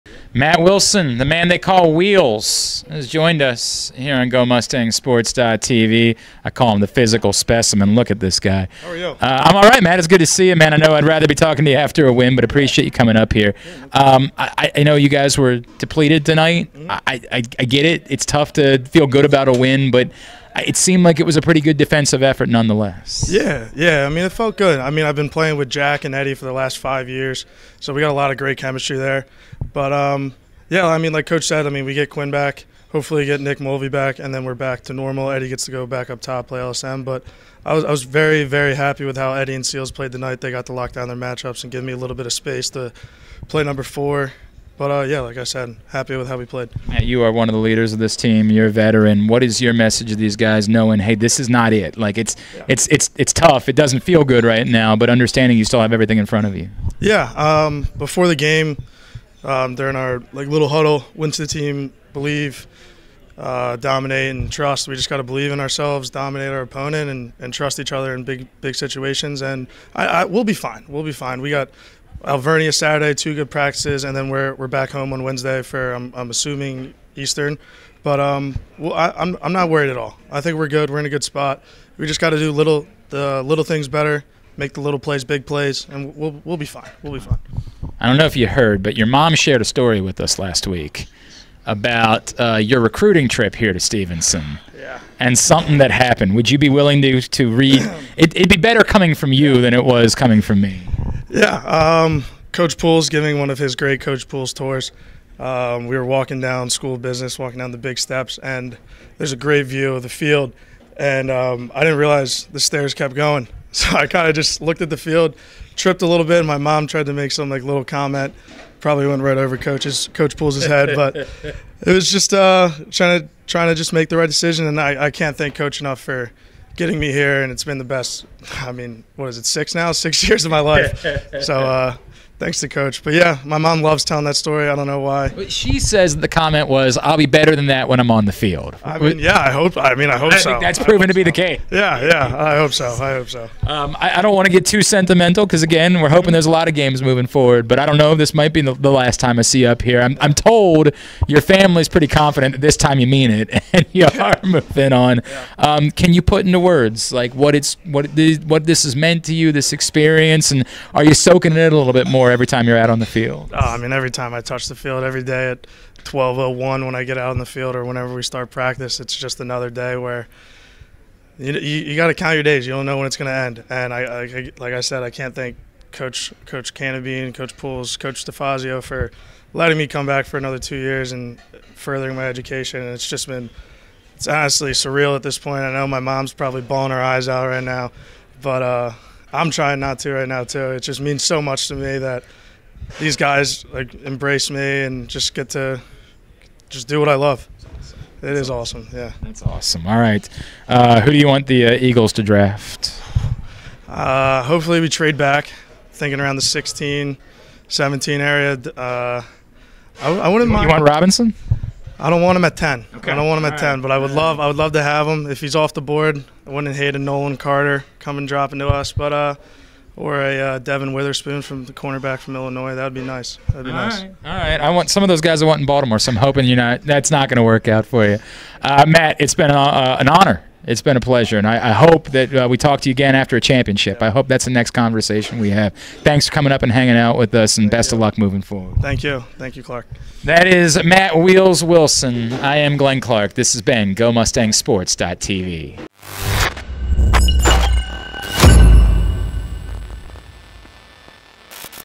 Men's Lacrosse vs. York (Pa.) | Postgame with Matt Wilson (4/26/2023)
Here's #10 graduate student defense Matt Wilson with final thoughts on a tough loss to the Spartans, as well as details on the regular season coming to a close as the team looks to host a semifinal game next Wednesday.